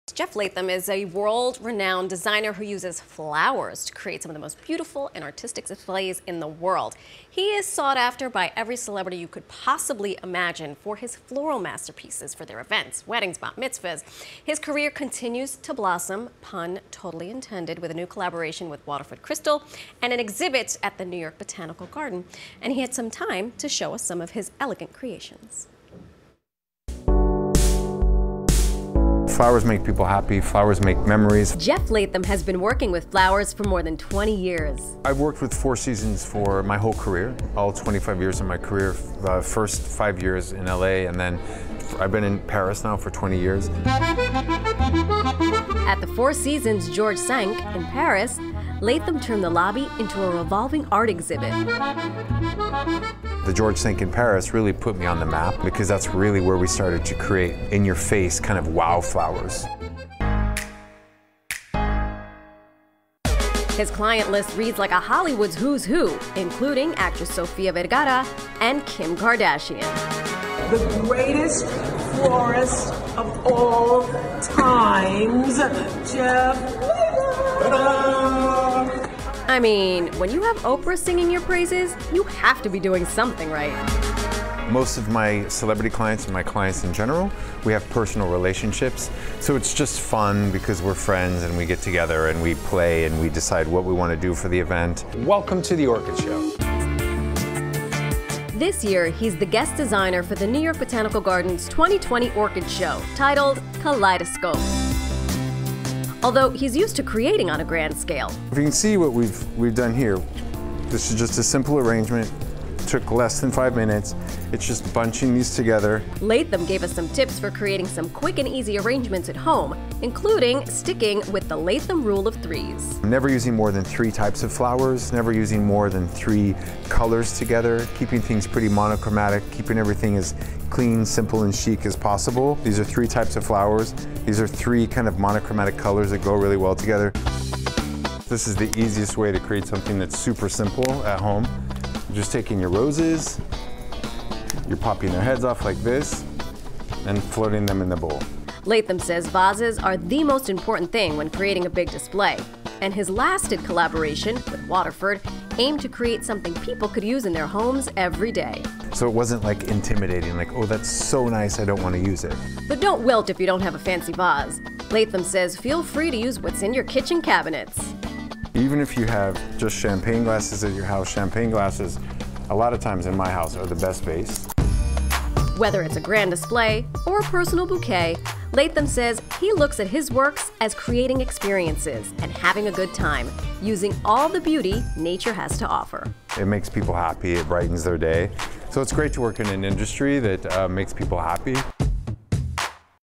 Flower arrangement tips from celeb florist Jeff Leatham
Celebrity florist Jeff Leatham talks about his career and shares some of his flower knowledge with PIX11's Marysol Castro. Plus, Leatham shares tips on how to create some simple-yet-beautiful flower arrangements that anyone can make at home.